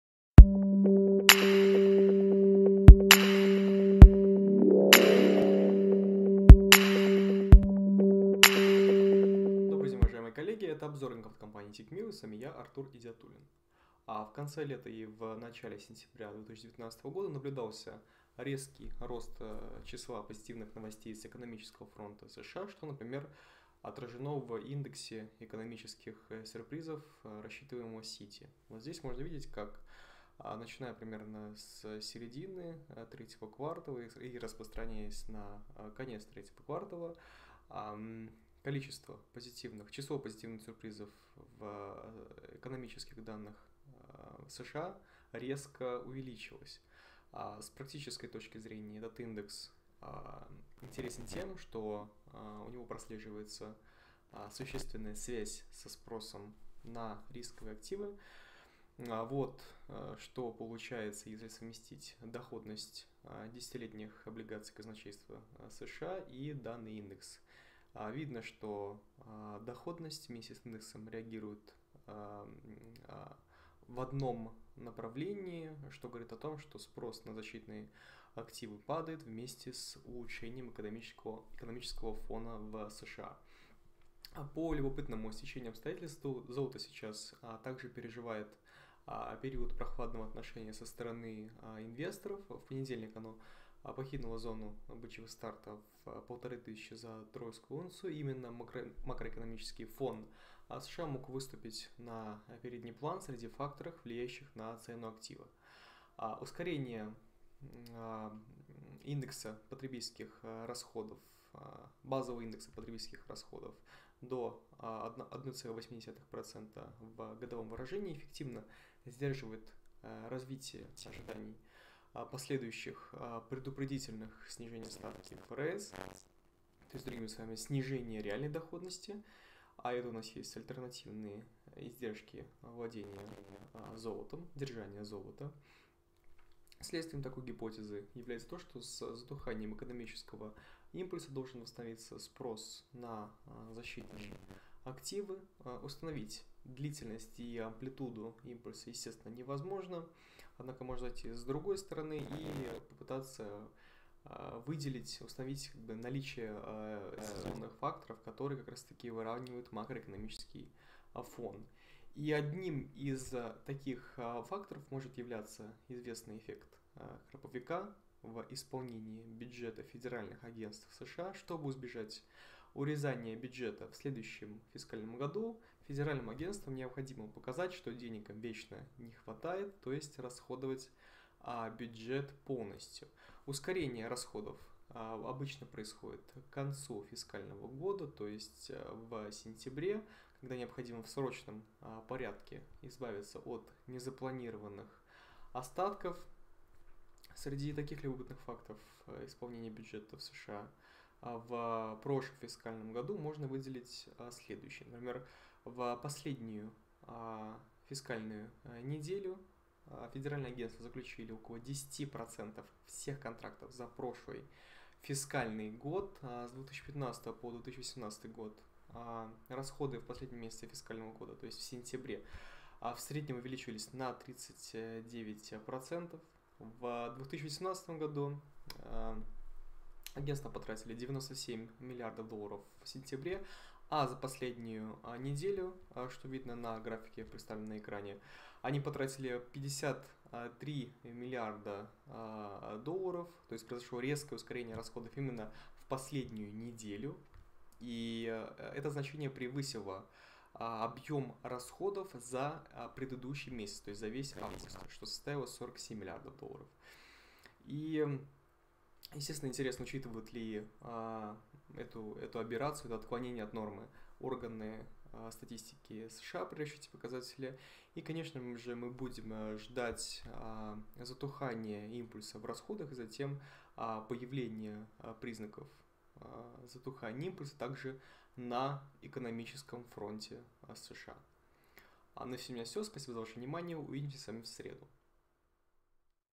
Старый добрый «эффект храповика» в исполнении бюджета США
В конце лета и в начале сентября наблюдался резкий рост числа позитивных новостей с экономического фронта США, что, например отражено в индексе экономических сюрпризов, рассчитываемого Citi.

Больше о Тикмилл:

3. Наш путь к успеху